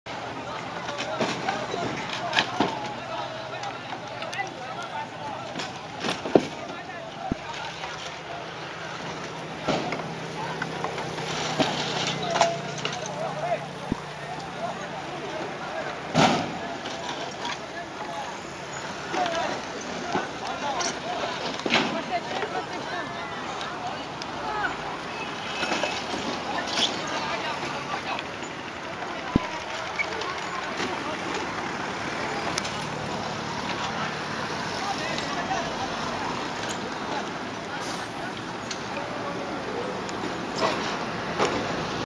Bamboo rafts loading
Loading bamboo rafts in trucks